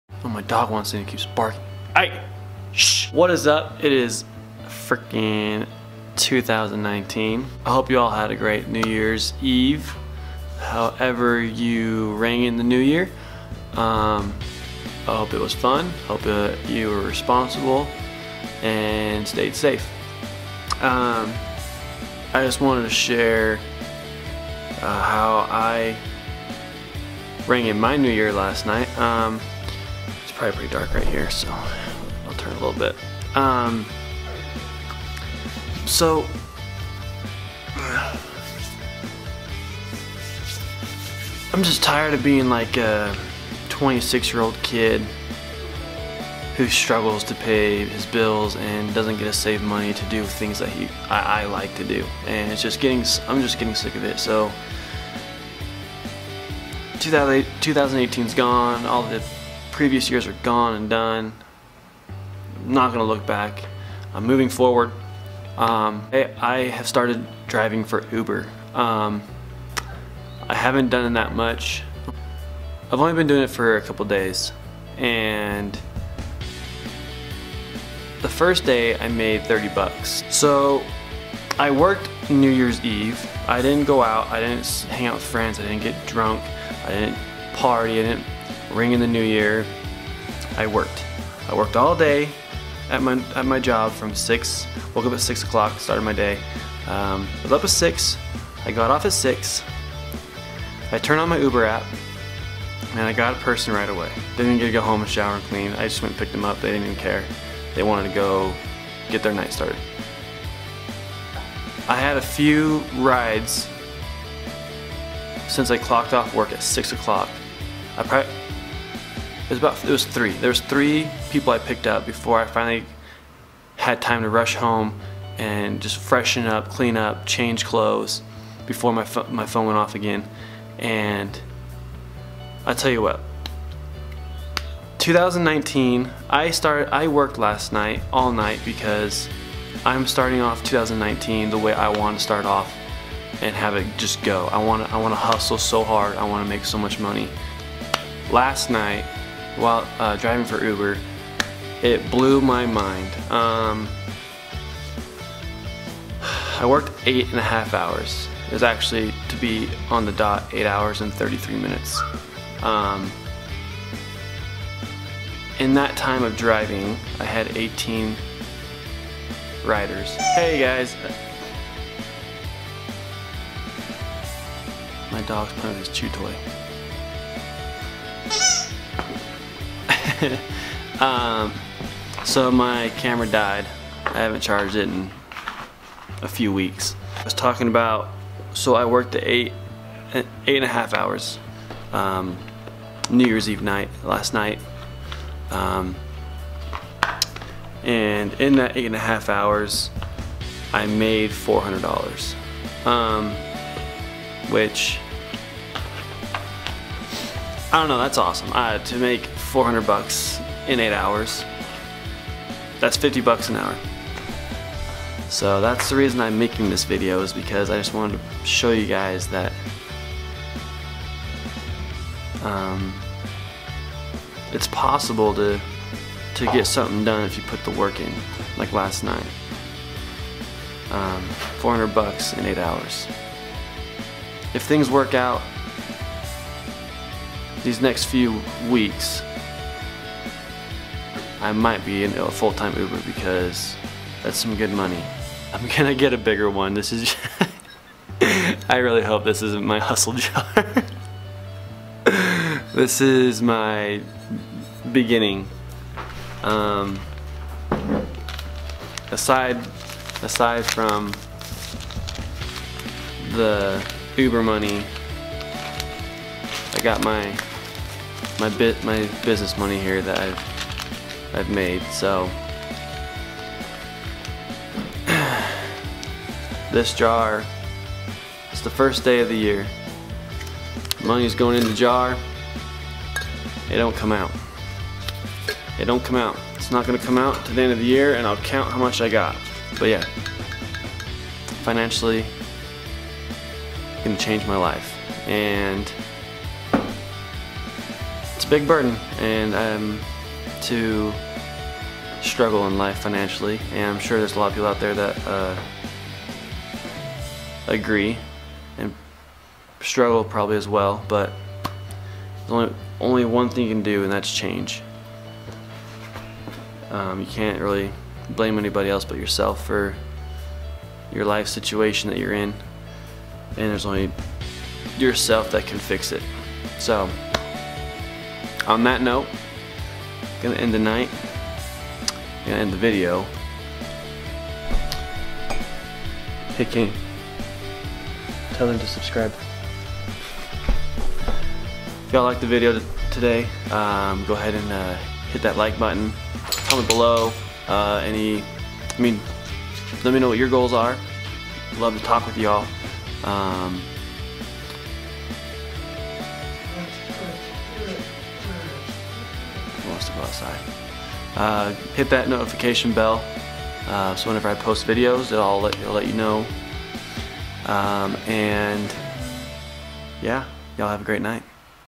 New Year, New Content
Yes, I’ve had this channel for a year or at least almost a year now but this is something completely different than I had originally planned and so I’m proud to say...

I’m a vlogger!!! 👏🏼🎉

This first video was random. I just decided to start recording about how I rang in my new year which was different than all these past years. I wouldn’t say it was a New Years resolution video, more along the lines of setting 1 particular goal in place. So yeah! Again welcome and I’m so excited for y’all to follow me along my journey! The few first months are going to be random videos, ride alongs, and Q&A’s until my surprise and true reason of vlogging occurs WHICH you will need to stick around to see what happens so until then I hope you enjoy my videos!!